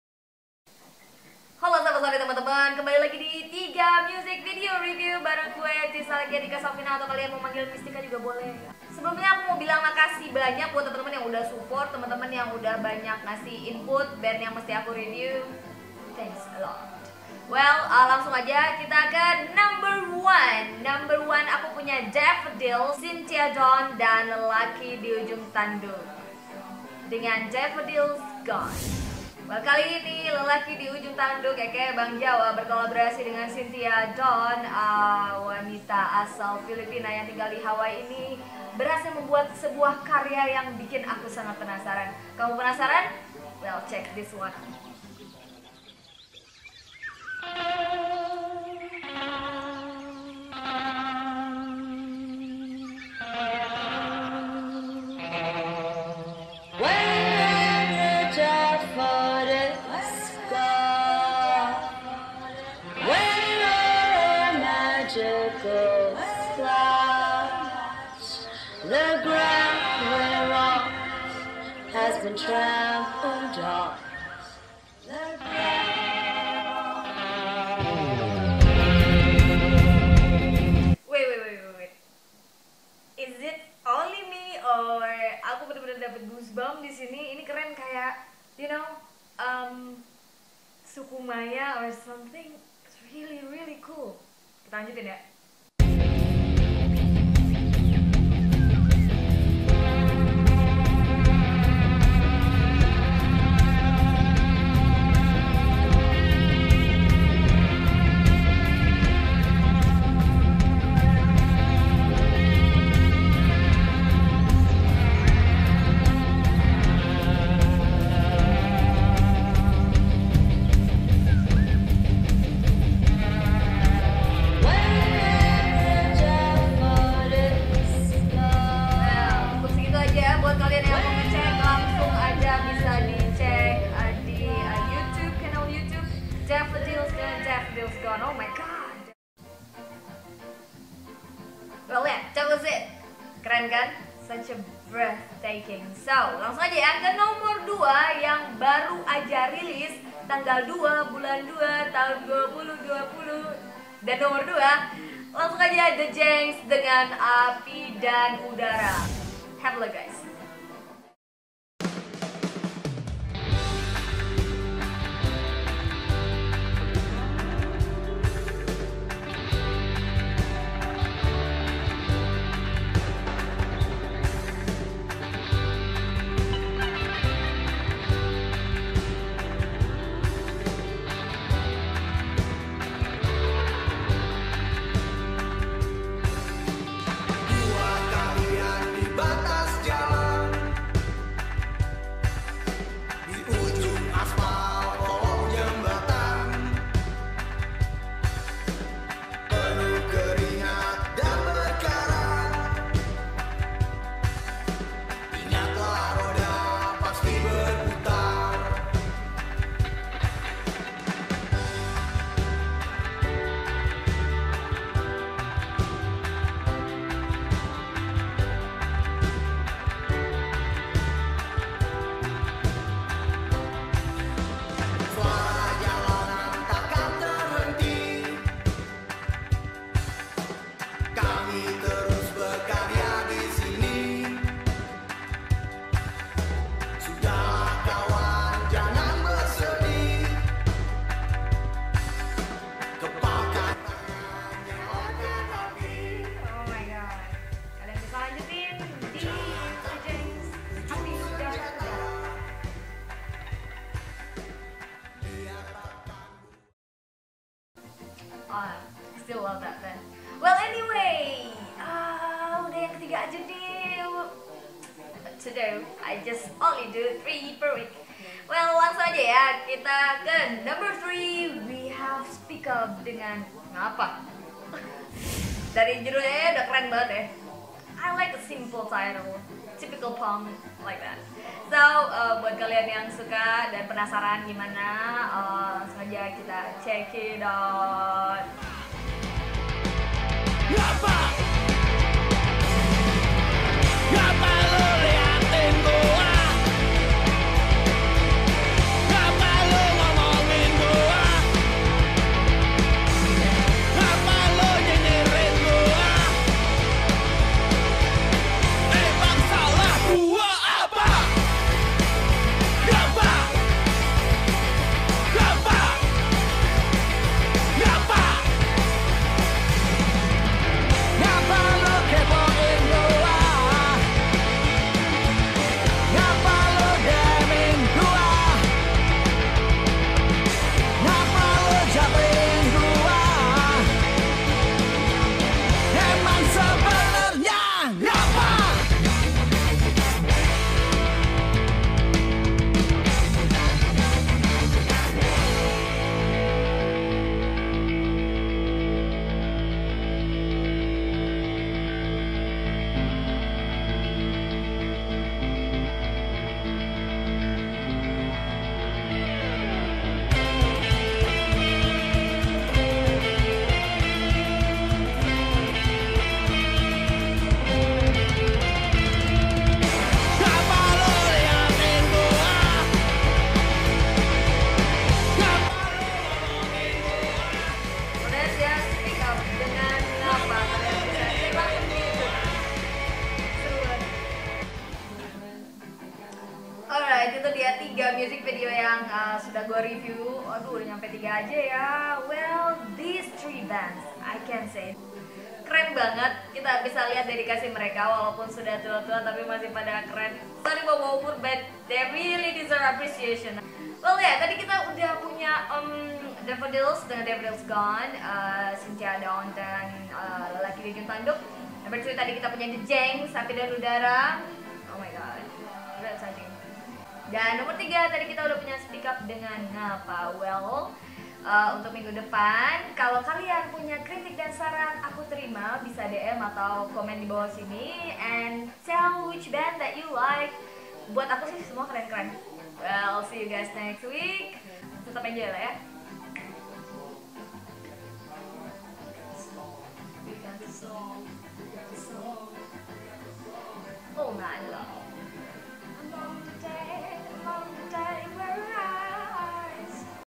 3 Music Video Review 07.02.2020
ayo ngintip music video band-band keren di Indonesia
Daffodils, Djenks dan Speak Up.